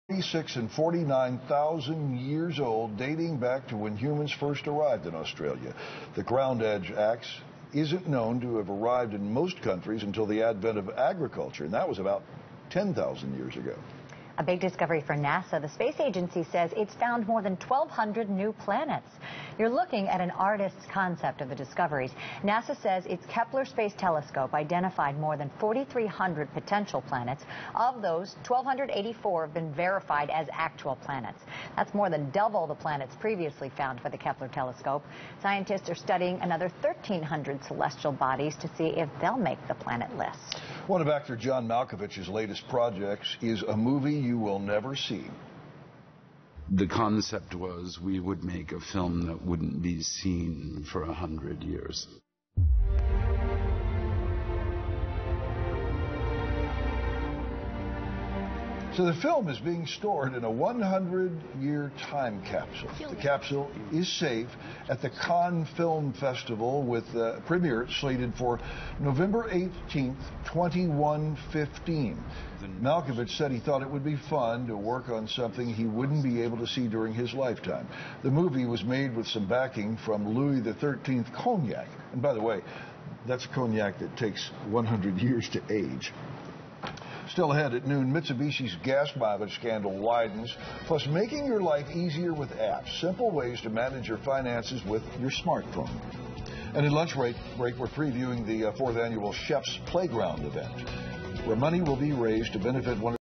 Latest News WGN | Headline News Nasa Spots 1,284 New Planets
- Headline News Nasa Spots 1,284 New Planets

Powered by, WGN Chicago